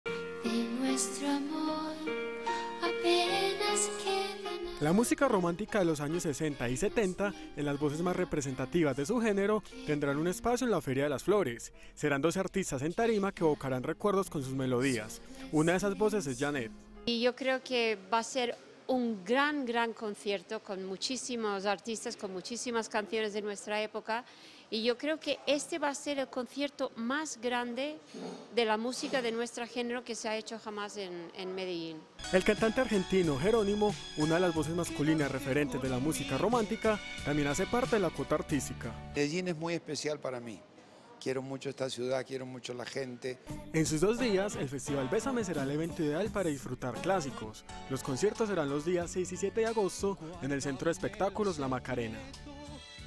La música romántica también tiene su espacio en la Feria de las Flores [Noticias] - TeleMedellin
La música romántica de los años 60 y 70 en las voces más representativas de su género,  tendrán un espacio en La feria de las flores. Serán 12 artistas en tarima que evocarán recuerdos con sus melodías. Una de esas voces es Jeanette.